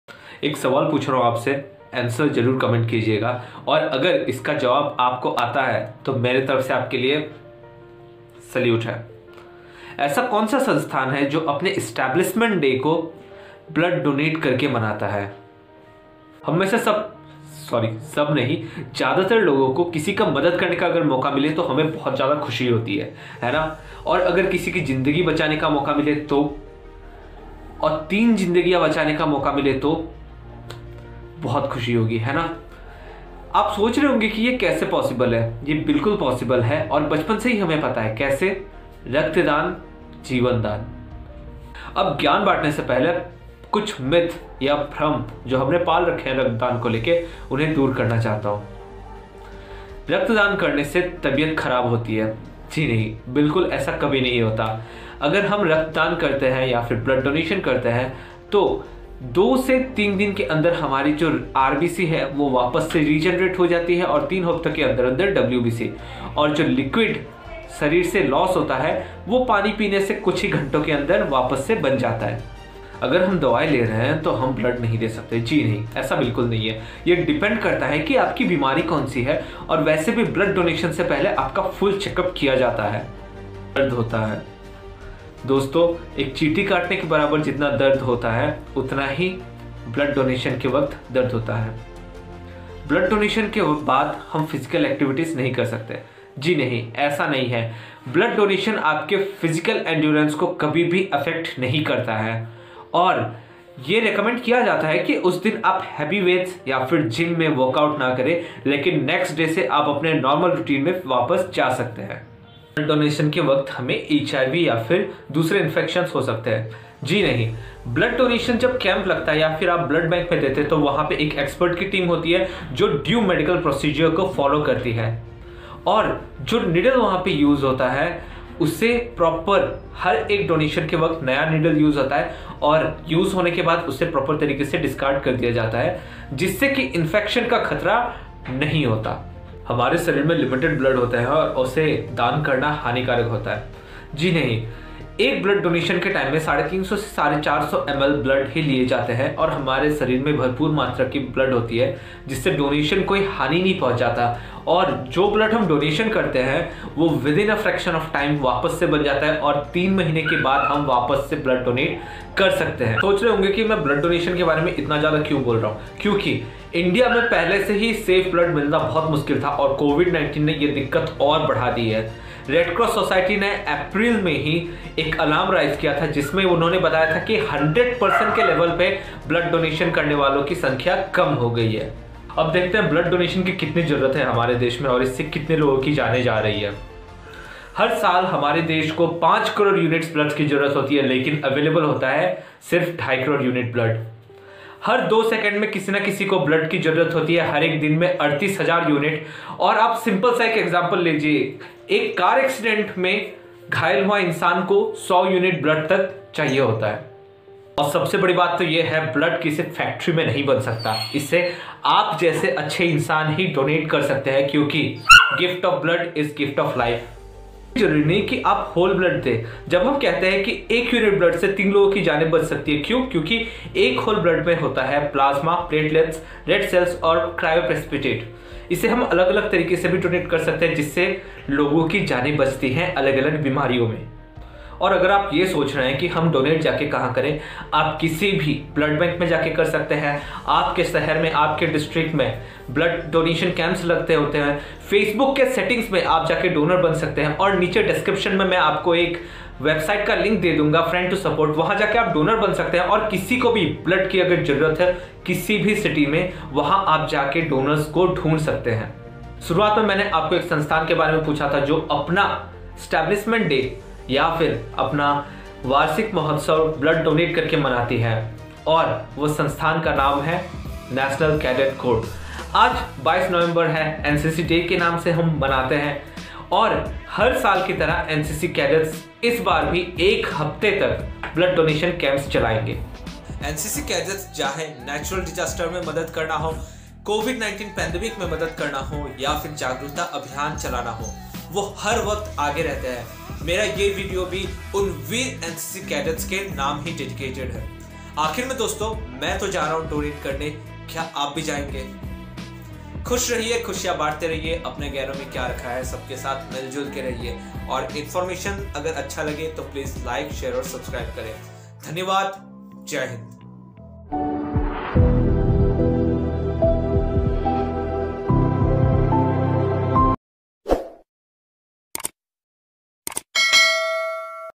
REQUIRED EVERY 2 SECS, DONATE BLOOD | NCC DAY SPECIAL 2020
This video showcases the need of blood donation in this time. Its dedicated to the VEER CADETS OF NCC. HAPPY NCC DAY.